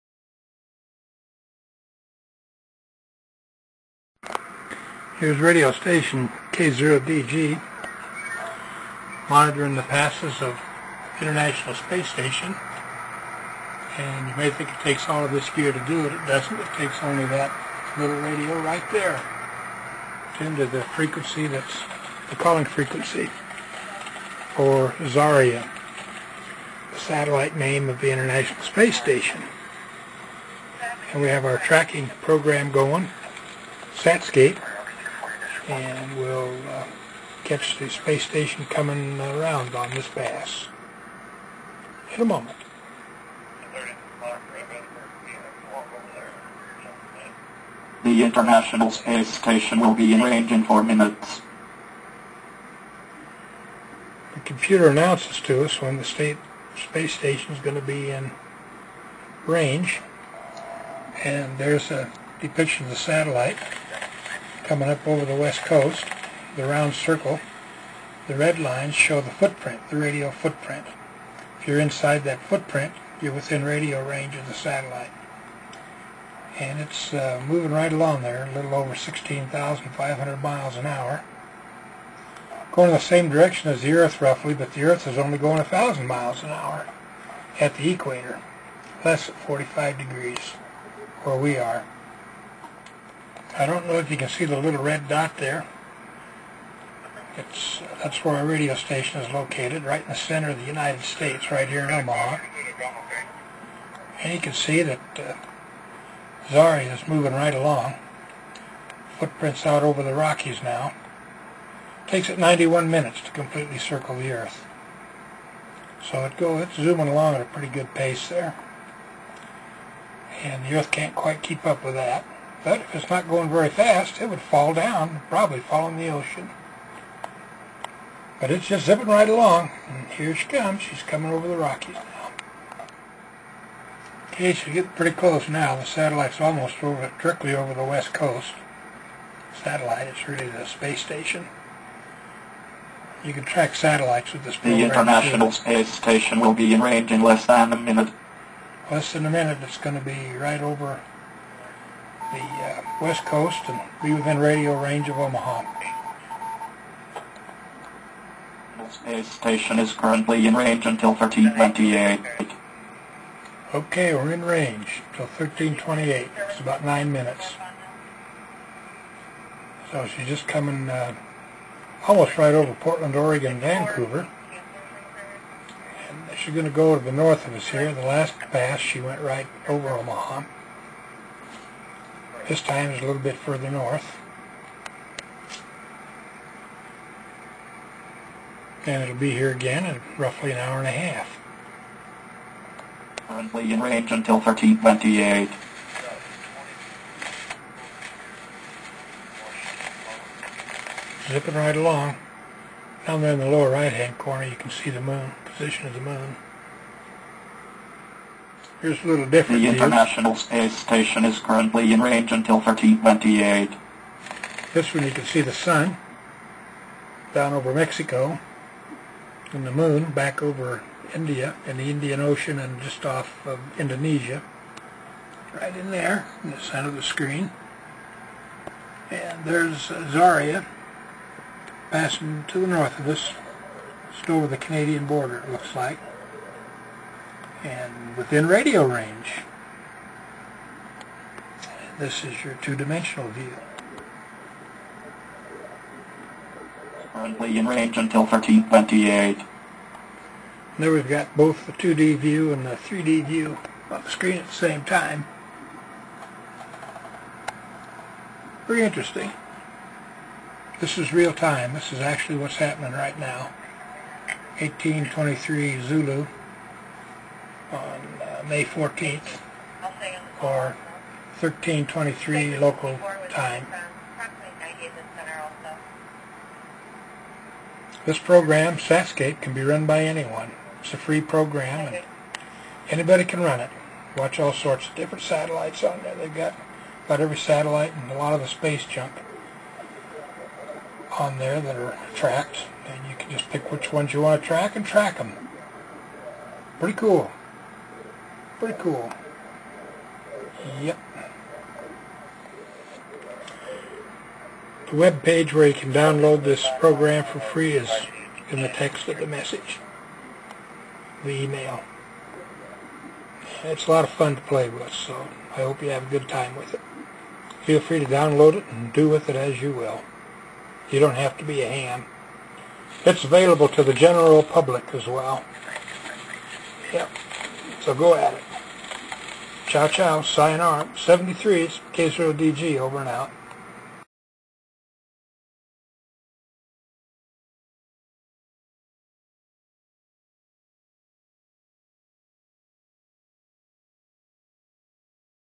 International Space Station (ISS) Tracking Demonstration
Using the SATSCAPE program by Scott Hather to track the International Space Station on a computer.